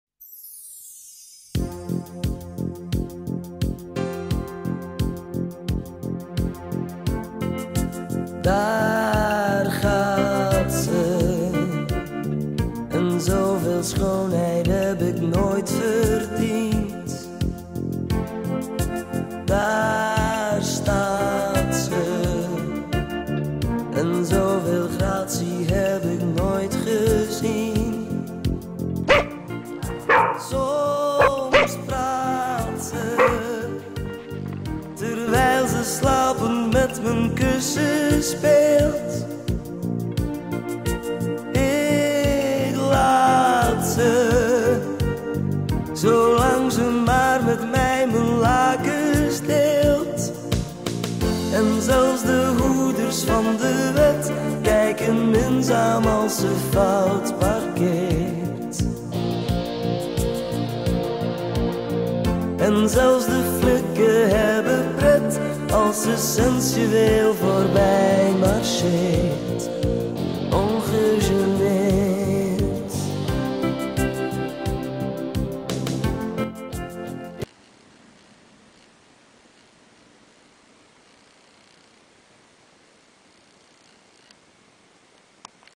My dog Zoë, a 10 year old Turkish lady
My first little movie, filmed with a fotocamera. Second movie coming up.